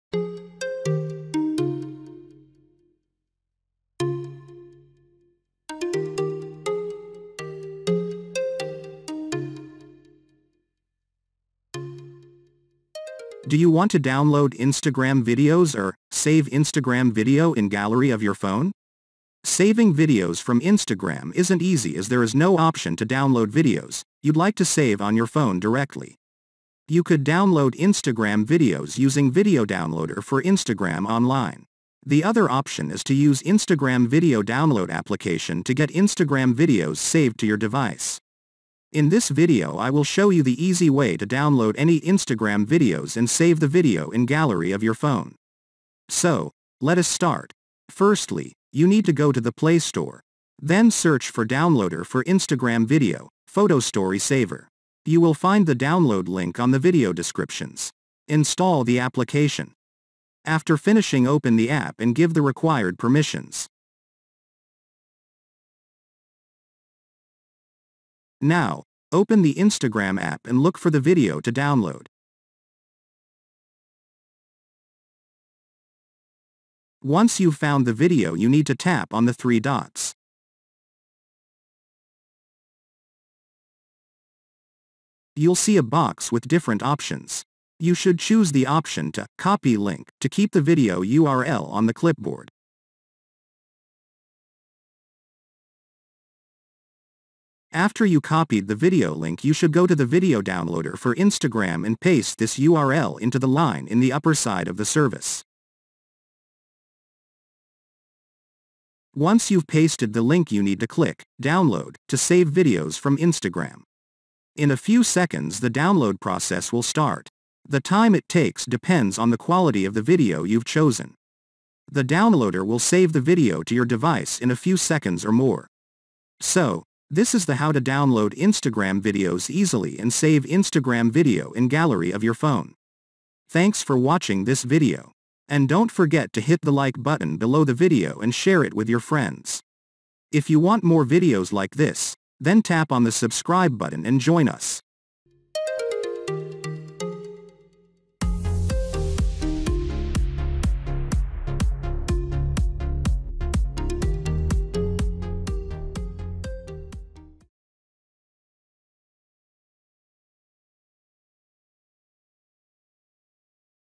How To Download Instagram Videos Easily Save Instagram Reels Video In Gallery!! - Howtosolveit
Do you want to Download Instagram Videos or, Save Instagram Video in Gallery of your phone? Saving videos from Instagram isn't easy as there is no option to download videos, you'd like to save reels on your phone directly. You could download Instagram videos using Video Downloader for Instagram online. The other option is to use Instagram Video Download application to get Instagram videos saved to your device. In this video I will show you How to Download any Instagram Videos easily and Save the Video in Gallery of your phone. Firstly, you need to go to the play store.
Then search for downloader for Instagram video, photo story saver.

Now, open the Instagram app and look for the video to download. Once you’ve found the video you need to tap on the three dots. You’ll see a box with different options. You should choose the option to “Copy link” to keep the video URL on the clipboard. After you copied the video URL you should go to the Video Downloader for Instagram and paste this URL into the line in the upper side of the service. Once you’ve pasted the URL you need to click “Download” to save videos from Instagram. In a few seconds the download process will start. The time it takes depends on the quality of the video you’ve chosen. The downloader will save the video to your device in a few seconds or more. So, this is the How to Download Instagram Videos easily and Save Instagram Video in Gallery of your phone.
See my other videos to find out more tips and tricks on Facebook and Whatsapp and other messengers apps.